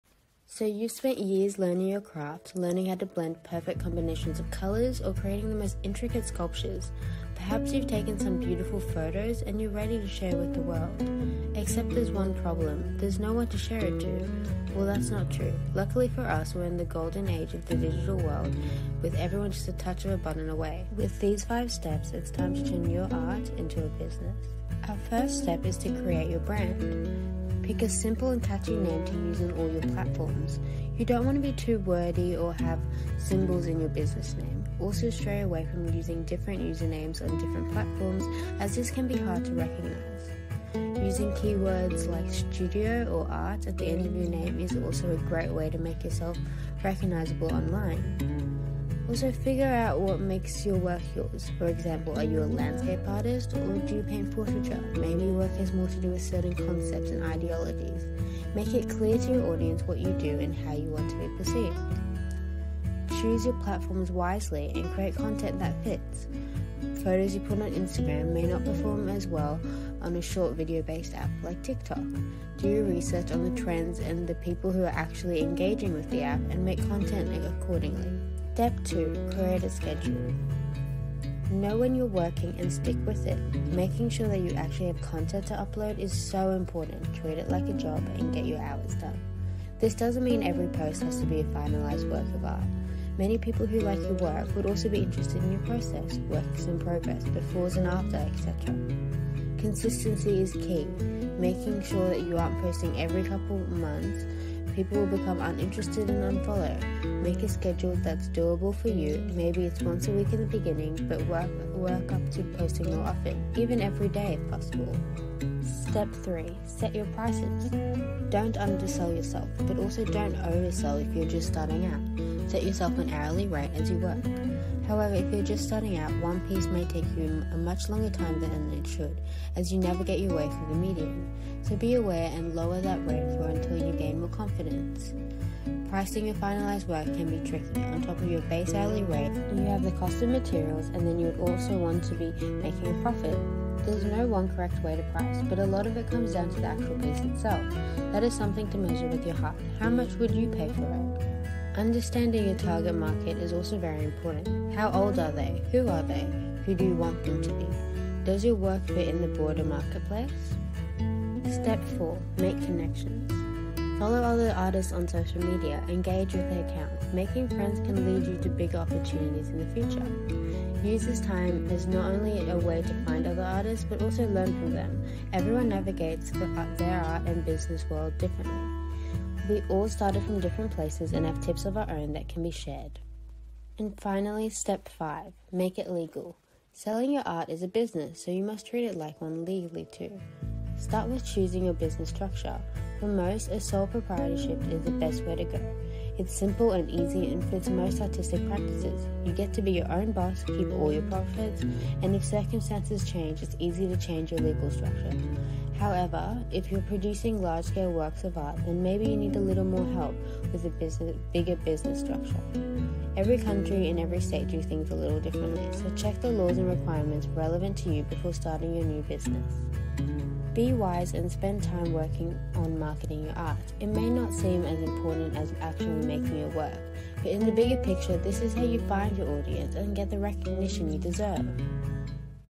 5 Steps To Turn your ART Into A Business | INTERNATIONAL INSTITUTE OF DIGITALMARKETING ™
You’ve spent years learning your craft, learning how to blend perfect combinations of colors, or creating the most intricate sculptures, perhaps you’ve taken some beautiful photos and you’re ready to share with the world.
Except there’s one problem. There’s no one to share it with. Well, that’s not true, Luckily for us, we’re in the golden age of the digital world with everyone just a touch of a button away. It’s time to turn your art into a business.

1. Create your brand
Pick a simple and catchy name to use on all your platforms. You don’t want to be too wordy or have underscores and other symbols in your business name. Also, stray away from using different usernames on different platforms as this can be hard to recognize.
Using keywords like ‘studio’ or ‘art’ to the end of your name is a great way to make yourself recognizable online.
Figure out what makes your work yours, for example, are you a landscape artist or do you paint portraiture? maybe your work has more to do with certain concepts and ideologies. Make it clear to your audience what you do and how you want to be perceived.
Choose your platforms wisely and create content that fits! Photos you put on Instagram may not perform well on a short video-based app like TikTok. Do your research on the trends and the people who actually engage with the app and make content accordingly. Marketing is about working with people not just about selling your products.
2. Create a schedule
Know when you’re working and stick with it – making sure you actually have content to upload is so important, treat it like a job and get your hours done. This doesn’t mean every post has to be a finalized work of art, many people who like your work would also be interested in your process, works in progress, before and afters, etc.
Consistency is key! – Make sure you aren’t posting once every couple of months, people will become uninterested and unfollow. Make a schedule that’s doable for you, maybe it’s once a week in the beginning but work up to posting more often, even every day.
Make sure you’re organized. If you’ve got an exhibition or a workshop coming up, make sure you have enough time to both create promotion and share it.
3. Set your prices
Don’t undersell yourself! But also don’t oversell if you’re just starting out! Set yourself an hourly rate as you work. However, if you’re just starting out, one piece may take you a much longer time than it should as you navigate your way through the medium, so be aware and lower that rate until you gain more confidence!
Pricing your finalized artworks can be tricky, on top of your base hourly pay you have the cost of materials and then you also would want to be making a profit. There’s no one correct way to price but a lot of it comes down to the actual piece itself, which is something to measure with your heart. How much would you pay for it?
Understand your target market – How old are they? Who are they? Who do you want them to be? Does your work fit in the broader marketplace?
When selling similar artworks make sure you’re consistent and fair. Don’t sell one for hundreds less than the other or at least have a good reason for it.
4. Make
Follow other artists, engage with their accounts. Making friends can lead you to bigger opportunities in the future.
You can even join artist groups on social media such as Facebook, it's a great way to meet and find opportunities local to you.
Use this time, not as just a way to find other artists but also to learn from them. Everyone navigates the art and business world differently; we all started from different places and have tips of our own that can be shared.
5. Make it
Selling your art is a business so you must treat it like one, legally too.
Start with choosing your business structure. For most, a sole proprietorship is the best way to go. It’s simple and easy and fits most artistic practices. You get to be your own boss, keep all your profits, and if circumstances change it's easy to change your legal structure. However, if you’re producing large-scale works of art then maybe you need a little more help with a bigger business structure.
Every country and every state do things a little differently, so check the laws and requirements relevant to you before starting your new business.
Be wise and spend time working on marketing your art, it may not seem as important as actually making your work, but in the bigger picture, this is how you find your audience and get the recognition you deserve!